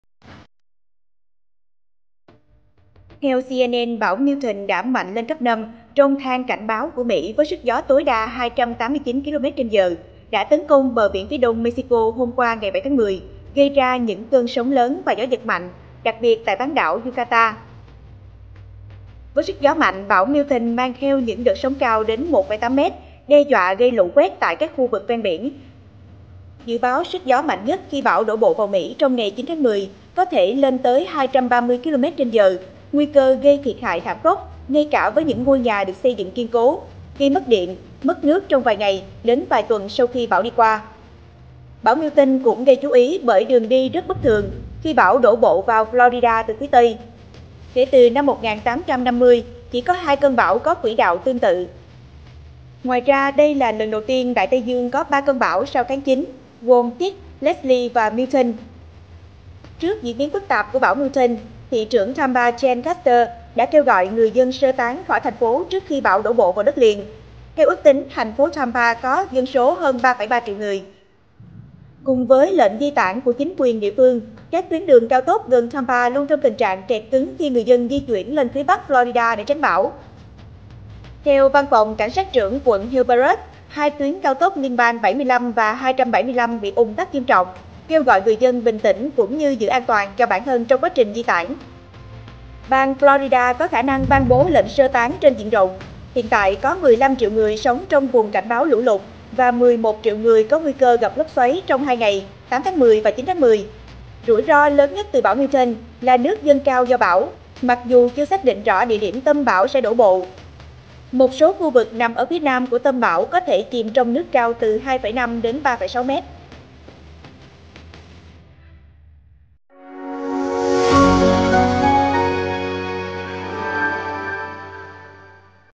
Bão Milton tấn công, bang Florida (Mỹ) sơ tán cư dân | Báo Phụ Nữ
Dự báo sức gió mạnh nhất khi bão đổ bộ vào Mỹ trong ngày 9/10 có thể lên tới 230 km/giờ, nguy cơ gây thiệt hại thảm khốc ngay cả với những ngôi nhà được xây dựng kiên cố; gây mất điện, mất nước trong vài ngày đến vài tuần sau khi bão đi qua.